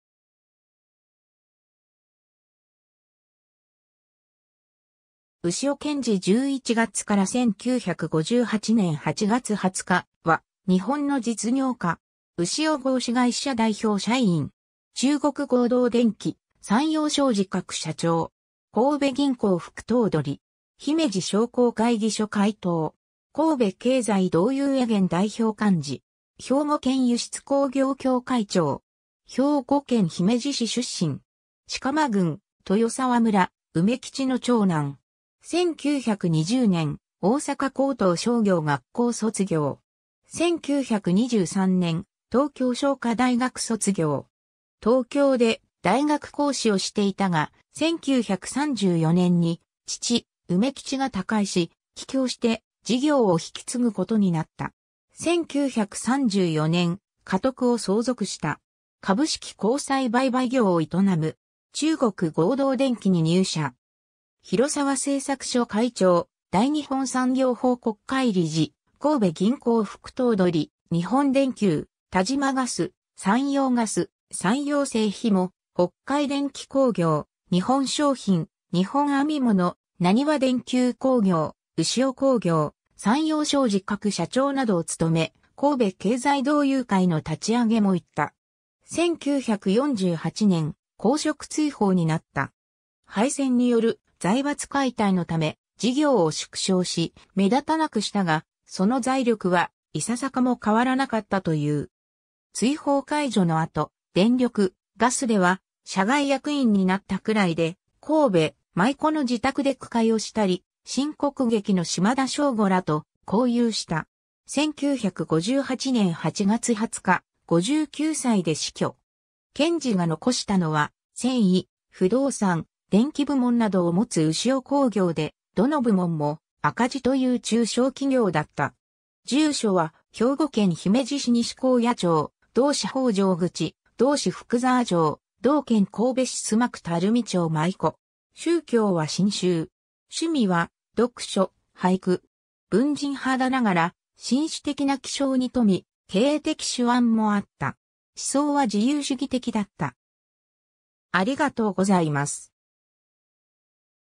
牛尾健治
牛尾 健治（うしお けんじ、1898年（明治31年）11月 - 1958年（昭和33年）8月20日）は、日本の実業家。
牛尾合資会社代表社員。
中国合同電気、山陽商事各社長。
神戸銀行副頭取。
姫路商工会議所会頭。
神戸経済同友会元代表幹事。
兵庫県輸出工業協会長。
 兵庫県姫路市出身。
飾磨郡豊沢村・梅吉の長男。
1920年、大阪高等商業学校卒業。
1923年、東京商科大学（現一橋大学）卒業。
東京で大学講師をしていたが、1934年に父・梅吉が他界し、帰郷して事業を引き継ぐことになった。
1934年、家督を相続した。
株式公債売買業を営む。
中国合同電気に入社。
 広沢製作所会長、大日本産業報国会理事、神戸銀行副頭取、日本電球、但馬瓦斯、山陽瓦斯、山陽製紐、北海電気興業、日本商品、日本編物、ナニワ電球工業、牛尾興業、山陽商事各社長などを務め、神戸経済同友会の立上げも行った。
 1948年、公職追放になった。
敗戦による財閥解体のため、事業を縮小し、目立たなくしたが、その財力は、些かも変わらなかったという。
追放解除のあと、電力、ガスでは社外役員になったくらいで、神戸・舞子の自宅で句会をしたり、新国劇の島田正吾らと交友した。
 1958年8月20日、59歳で死去。
健治が残したのは、繊維、不動産、電機部門などをもつ牛尾工業で、どの部門も赤字という中小企業だった。
 住所は兵庫県姫路市西紺屋町・同市北条口・同市福沢町、同県神戸市須磨区垂水町舞子。
宗教は真宗。
趣味は読書、俳句。
文人肌ながら、進取的な気象に富み、経営的手腕もあった。
思想は自由主義的だった。